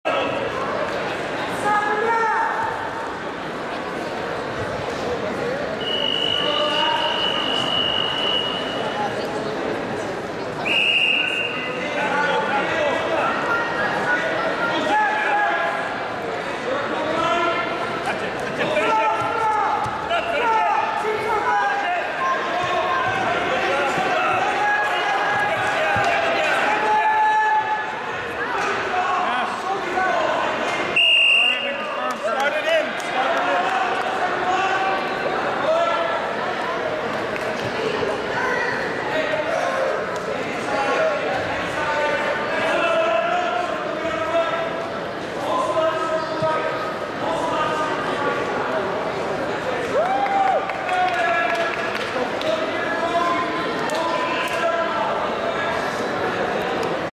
2014 Ont Juvenile Prov FW 46kg Erica Turcott (Sudbury) vs Sarah Hein (Wildcat)
Ontario Juvenile Provincial Wrestling Championship.  February 1,2, 2014, Sudbury, Ontario, Canada. FW 46kg